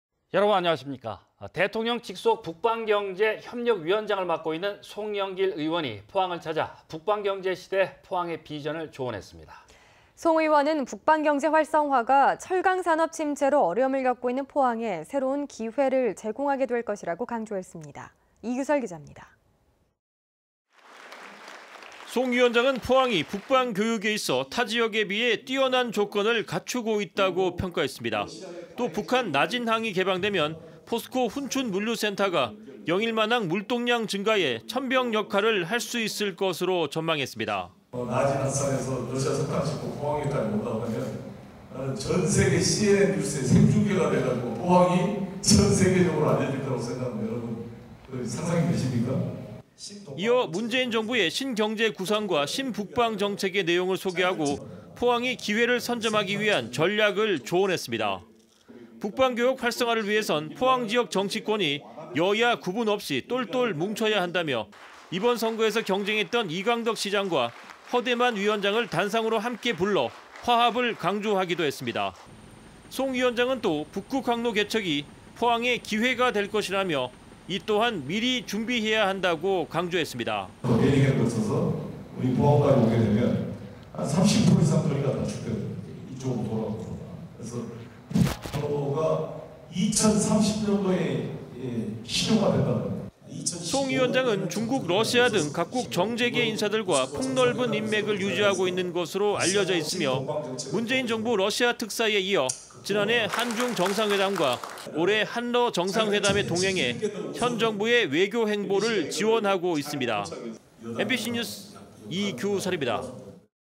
[포항MBC뉴스]송영길 의원 "포항에 분명 기회 온다!"-이규설
◀ＡＮＣ▶
대통령 직속 북방경제협력위원장을 맡고 있는
송영길 의원이 포항을 찾아
북방경제 시대 포항의 비전을 조언했습니다.

송 의원은 북방경제 활성화가
철강산업 침체로 어려움을 겪고 있는 포항에
새로운 기회를 제공하게 될 것이라고

이규설 기잡니다.
           ◀ＥＮＤ▶
           ◀ＶＣＲ▶

송 위원장은 포항이 북방교역에 있어
타지역에 비해 뛰어난 조건을 갖추고
있다고 평가했습니다.

또 북한 나진항이 개방되면
포스코 훈춘 물류 센터가
영일만항 물동량 증가에 첨병 역할을 할 수
있을 것으로 전망했습니다.

  ◀ＩＮＴ▶송영길 의원/북방경제협력위원장
0337 "나진 하산에서 러시아 석탄을 싣고 포항까지 온다면 전세계 CNN뉴스에 생중계가 되어서 포항이 전세계적으로 알려진다고 생각합니다. 여러분 상상이 되십니까?"

이어 문재인 정부의 신경제 구상과 신북
방정책의 내용을 소개하고 포항이
기회를 선점하기 위한 전략을 조언했습니다.

북방교역 활성화를 위해선
포항지역 정치권이 여· 야 구분없이
똘똘 뭉쳐야 한다며
이번 선거에서 경쟁했던 이강덕 시장과
허대만 위원장을 단상으로 함께 불러
화합을 강조하기도 했습니다.

송 위원장은 또 북극항로 개척이 포항의
기회가 될 것이라며 이 또한 미리 준비해야
한다고 강조했습니다

2144 "베링해를 거쳐서 우리 포항까지 오게되면 30%이상 거리가 단축됩니다.북극 항로가 2030년도에 실용화된다는 겁니다"

송 위원장은 중국, 러시아 등
각국 정·재계 인사들과 폭넓은 인맥을
유지하고 있는 것으로 알려져 있으며,

문재인 정부 러시아 특사에 이어
지난해 한중정상회담과 올해 한러정상회담에
동행해 현 정부의 외교 행보를
지원하고 있습니다.

MBC뉴스 이규설 입니다.